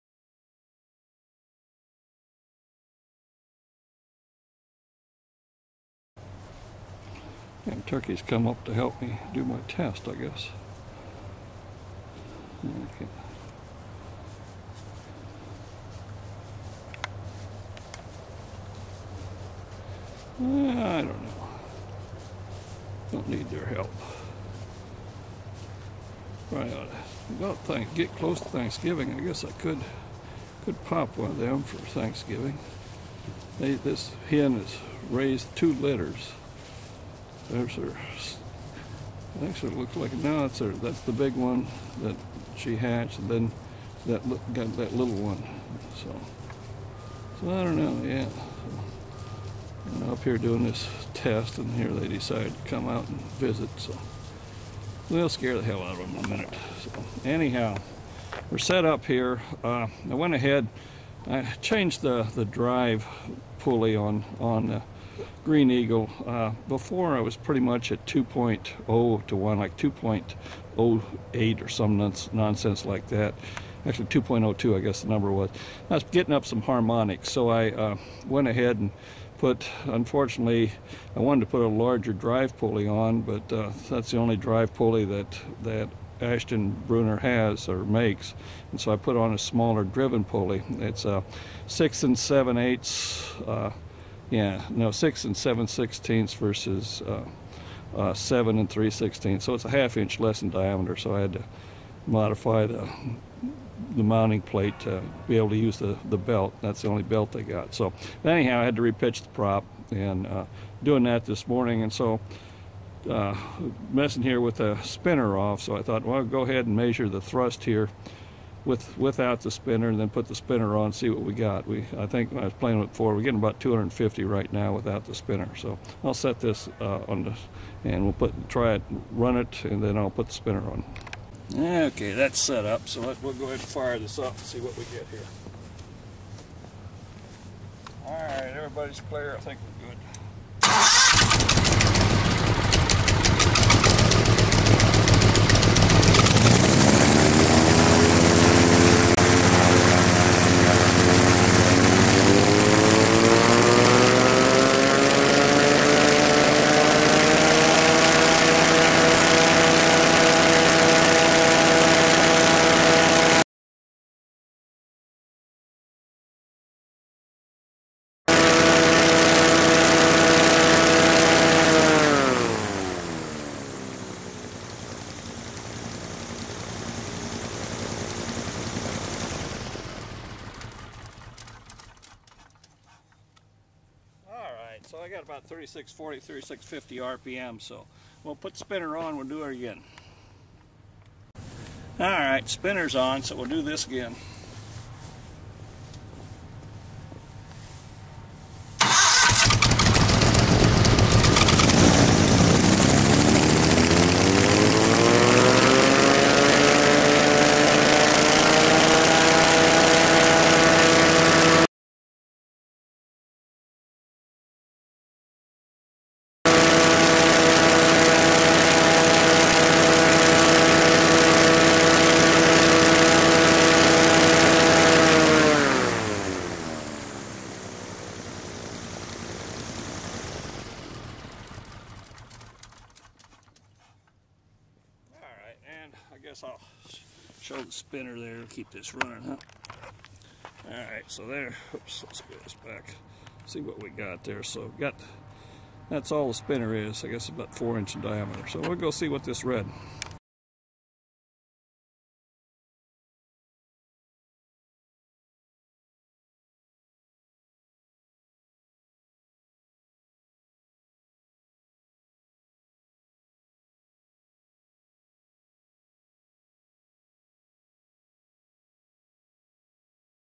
Propeller Spinner Thrust Test
Test to measure the increase in thrust from adding a spinner to a pusher propeller.